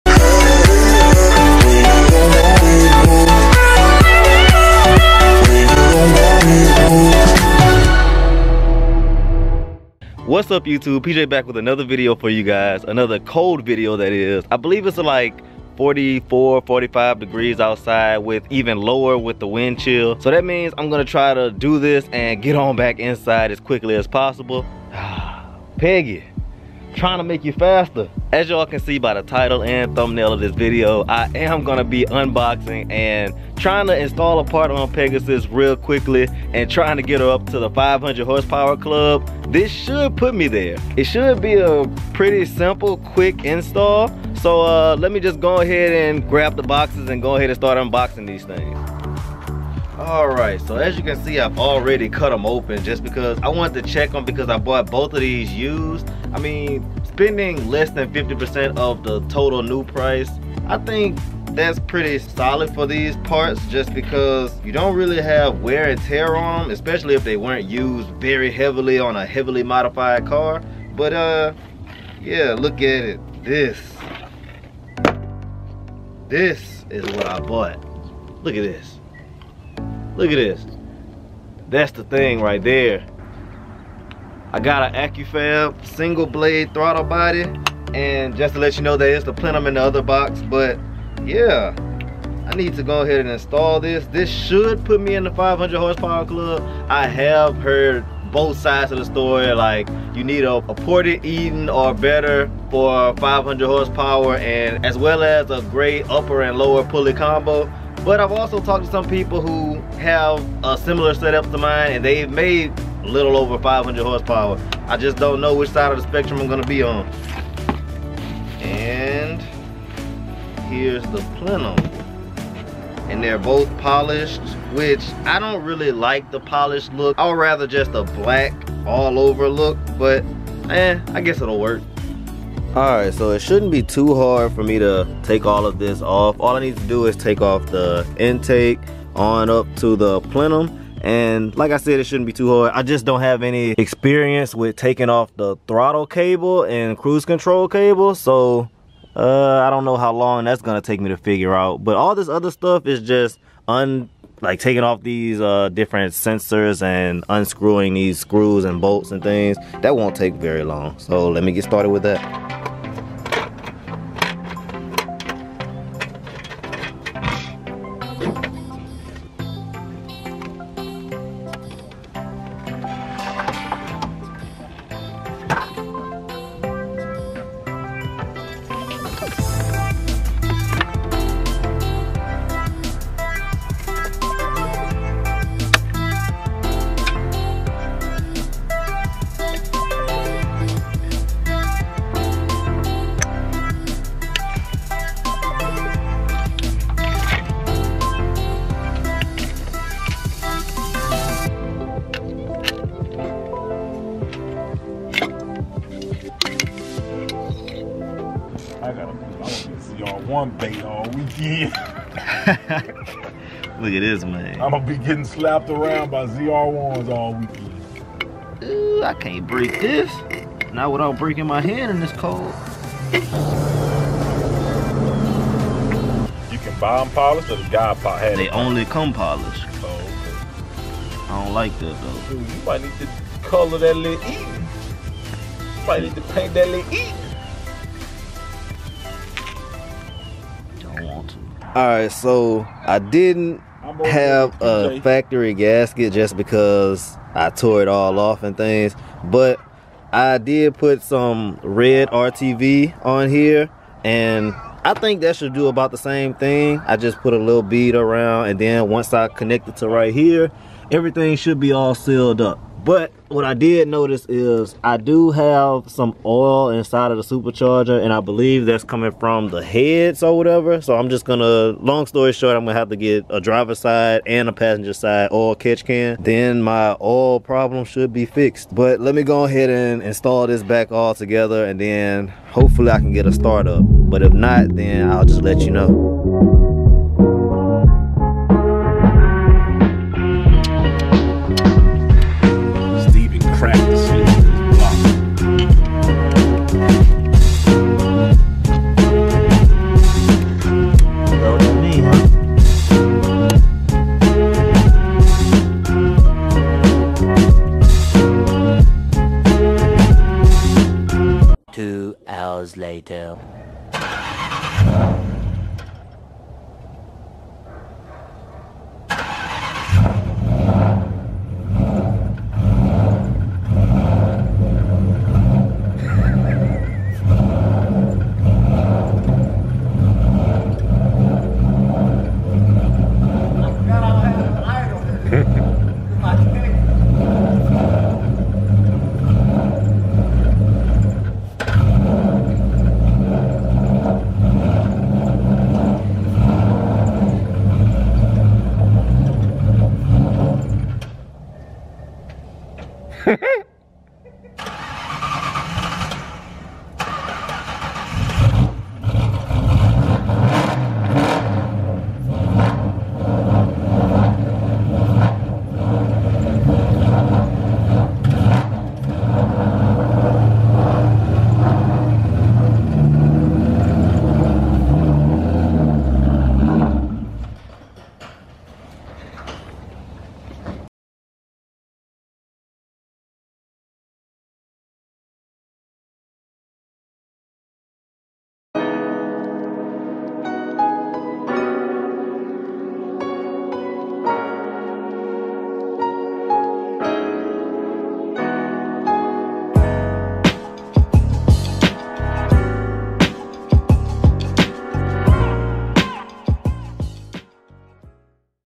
Do I FINALLY Have a 500WHP TERMINATOR COBRA.!? (Throttle Body Install)
Do I finally have a 500whp Terminator Cobra? Have I finally achieved 500hp?

By me installing this Accufab Single Blade Throttle Body and Plenum, I should be at or around the 500hp mark. Let me know your opinions!

PERFORMANCE MODS :
LoudMouth1 Catback
BBK O/R X-Pipe
JLT CAI
2.7in Upper Pulley
100mm Auxiliary Idler
170 Degree Thermostat
Accel Super Coil Packs
Accufab Single Blade Throttle Body/Plenum
DiabloSport MAFia
Kenne Bell Boost-A-Pump
Custom Bama Tune